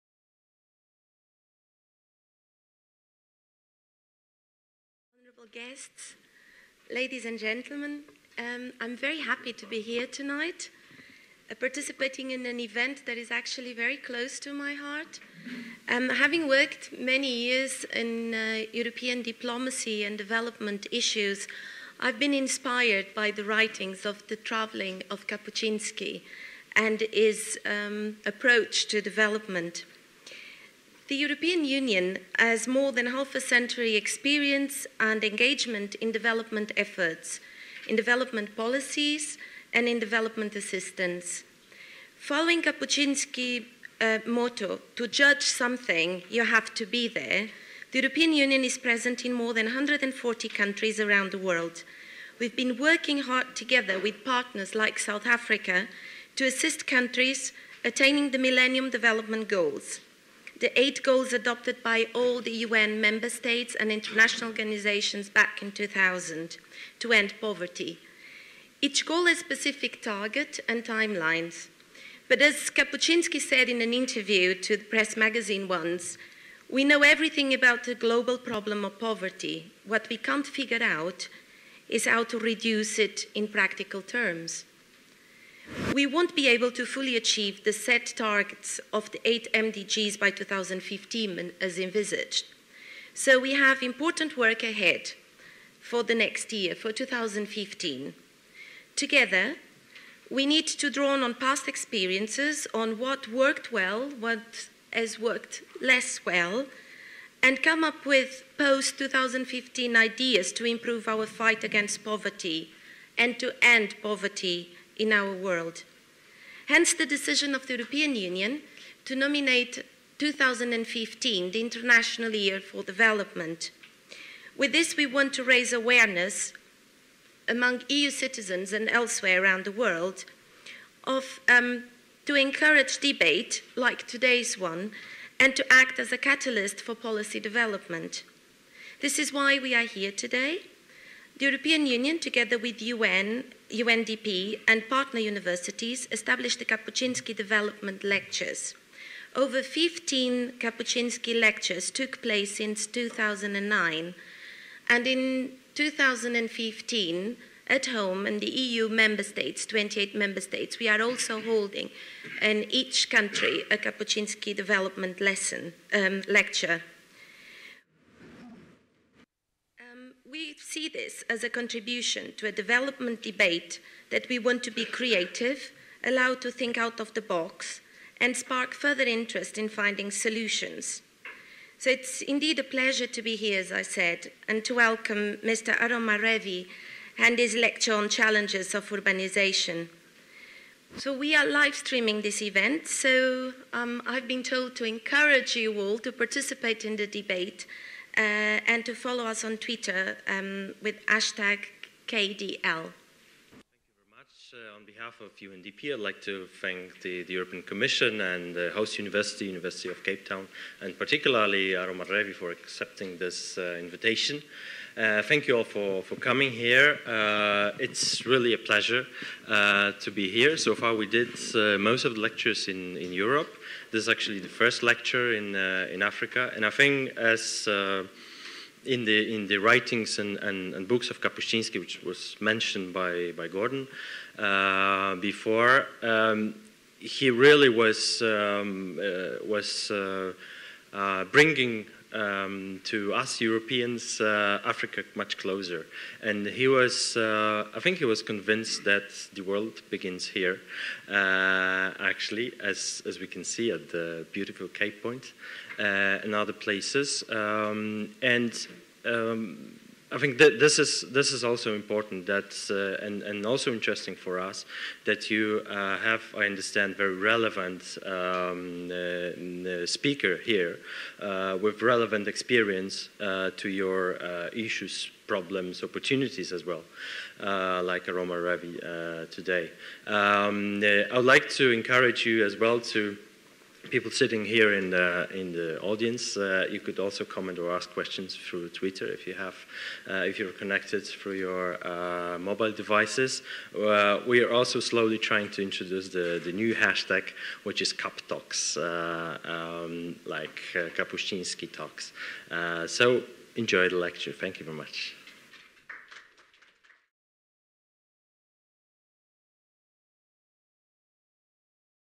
Kapuscinski Development Lecture | Introduction to Aromar Revi, IIHS
Sources:
University of Cape Town South Africa

UCT hosted a high-level Kapuscinski Development Lecture on Wednesday 5 November 2014, delivered by Aromar Revi, Director of the Indian Institute of Human Settlements and leading expert on global environmental change.

Organised jointly by the United Nations Development Programme, the European Commission, the African Centre for Cities and the University of Cape Town, Revi's lecture – titled "Putting the Urban at the Heart of the 2030 Sustainable Development Goals" – addressed the challenges African and Asian countries are facing with urbanisation.